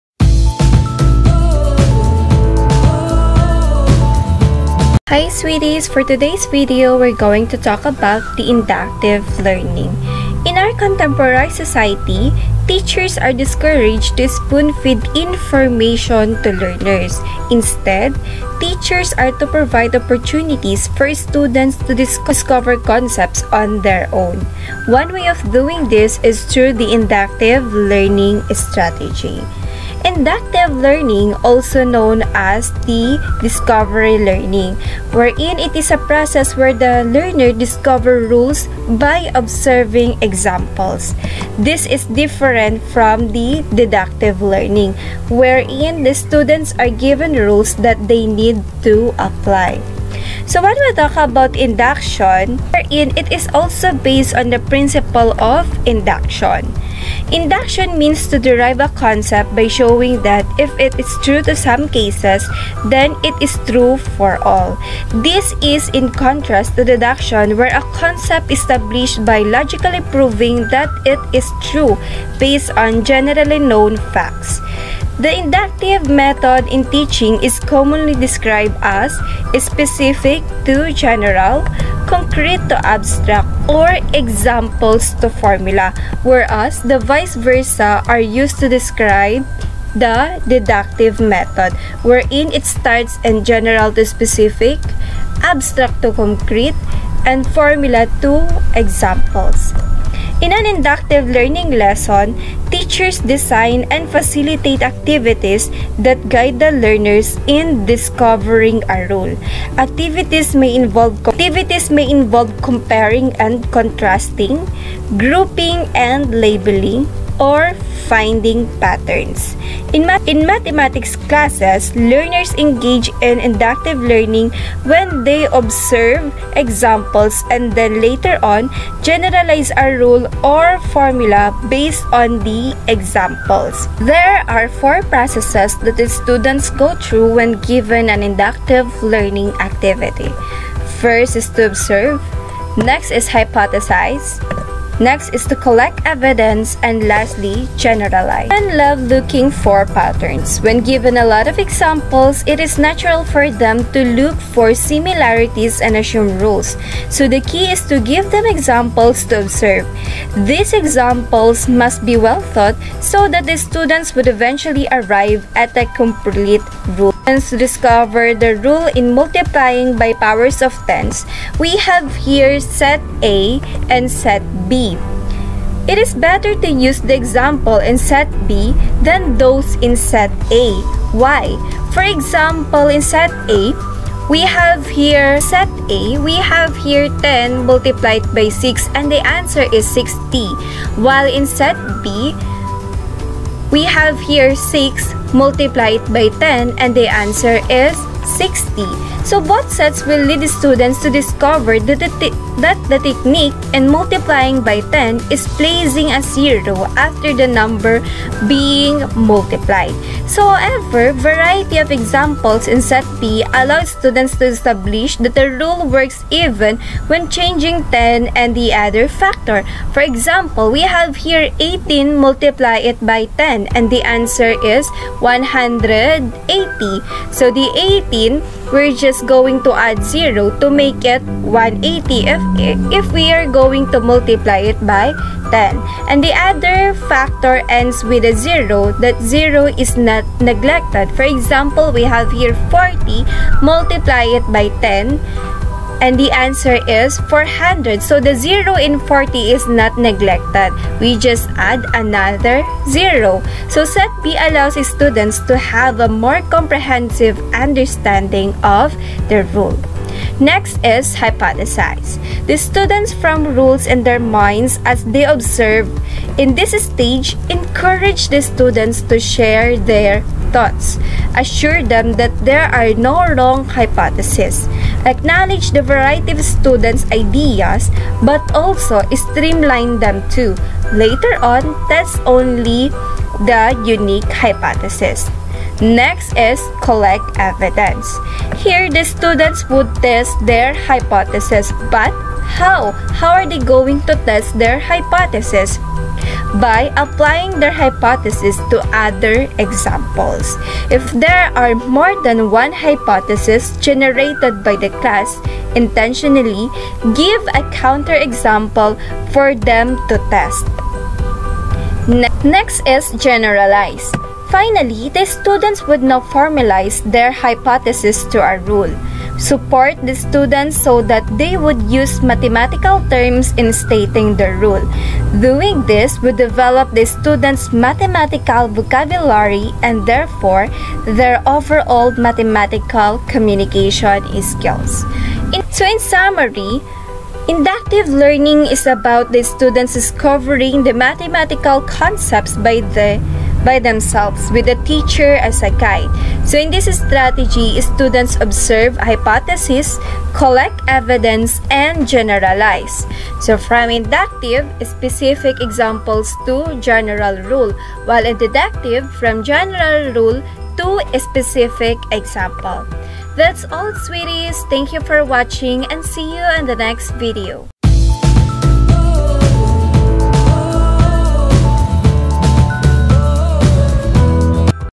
INDUCTIVE LEARNING (DISCOVERY LEARNING)
DISCOVERY LEARNING

TEACHERS PROVIDE OPPORTUNITIES FOR STUDENTS TO DISCOVER CONCEPTS ON THEIR OWN